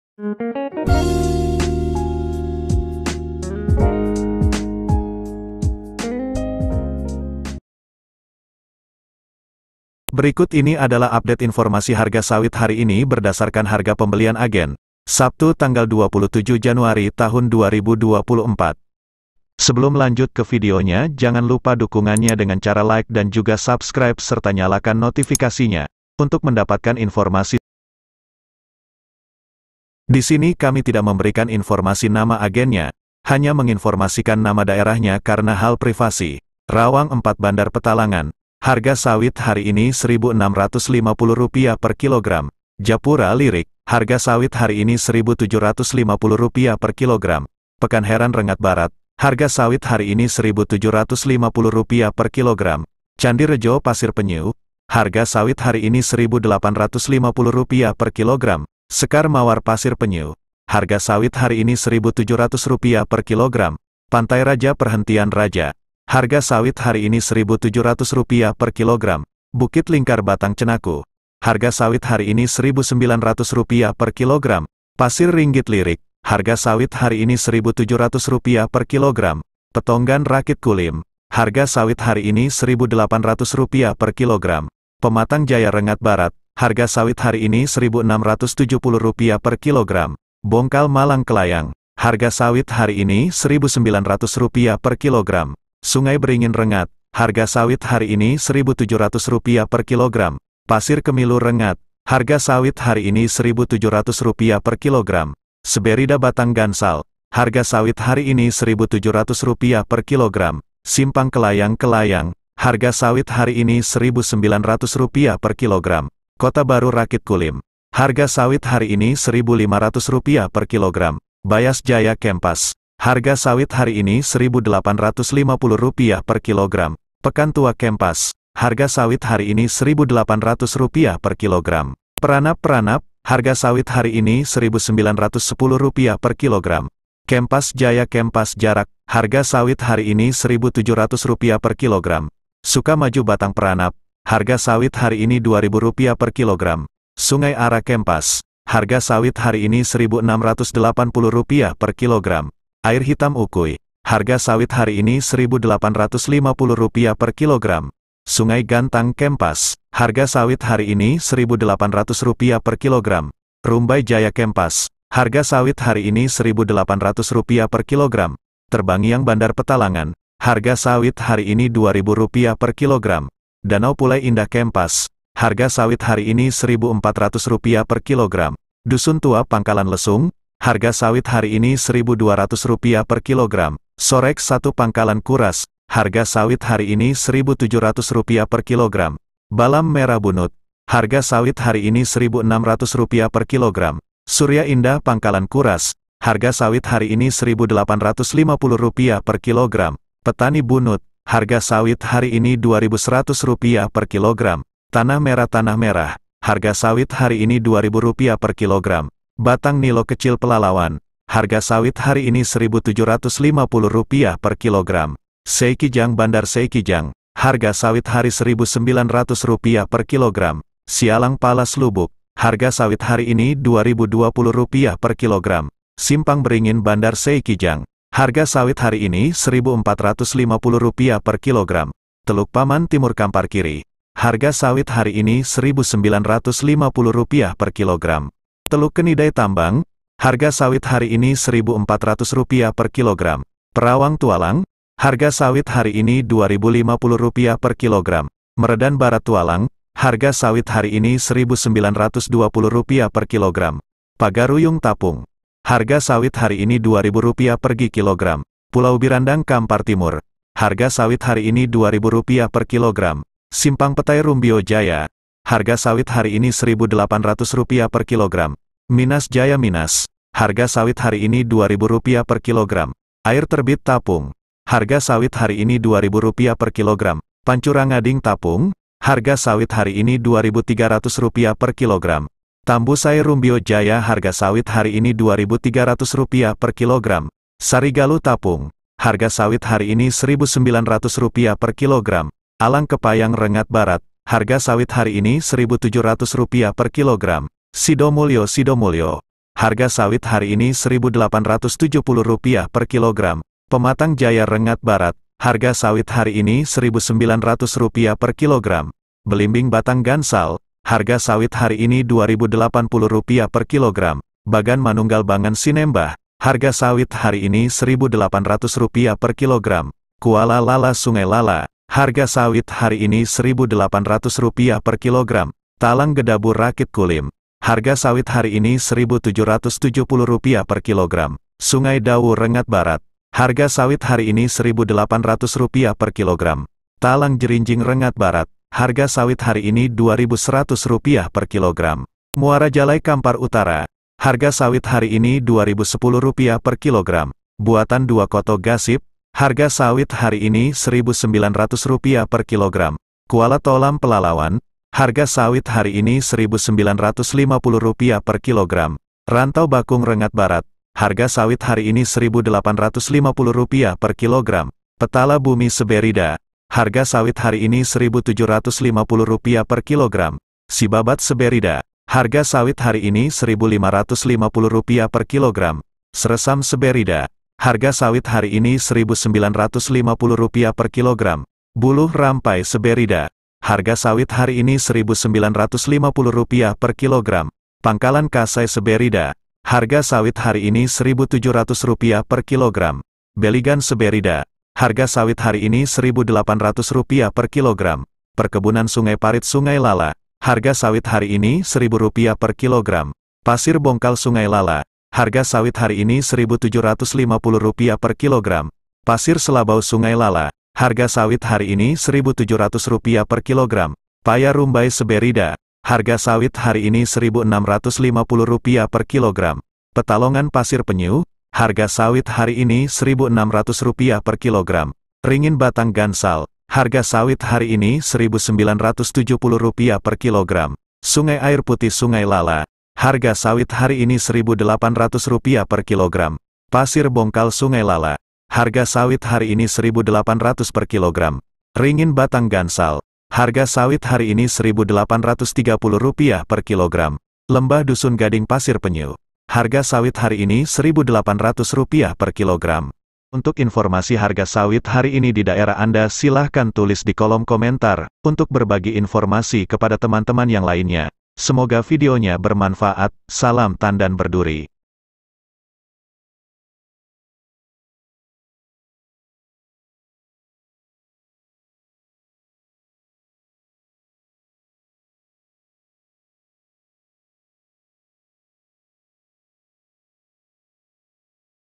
HARGA SAWIT HARI INI DI AGEN - TERBARU 27 JANUARI 2024
Berikut Ini Adalah Update informasi Harga Sawit Hari Ini Berdasarkan Harga Pembelian agen, Sabtu 27 Januari 2024.

Disini kami Tidak Memberikan Informasi Nama agennya, Hanya Menginformasikan Nama
Daerahnya Karena Hal Privasi.